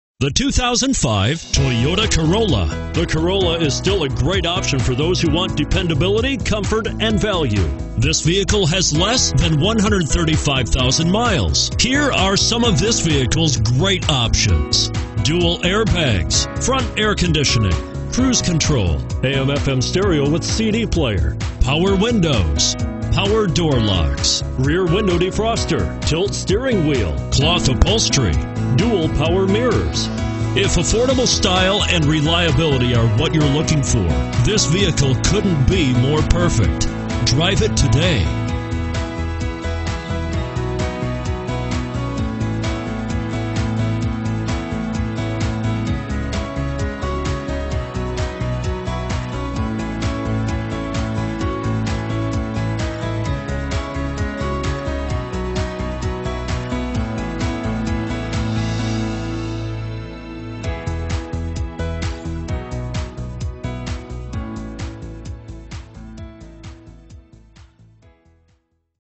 2005 Toyota Corolla Frankfort, Chicago, Bolingbrook 38139
2005 Toyota Corolla LE

Don't let this awesome 2005 Toyota Corolla  LE get away, with luxuries like CD player.  This vehicle is one of the safest you could buy. It earned a safety rating of 4 out of 5 stars.  Now you can be your own meteorologist with climate control.  The CD player keeps your favorite musicians at your fingertips.  According to a review from New Car Test Drive, 'Smooth and refined, practical and reliable'.  Could this be the vehicle for you? Give us a call today and find out!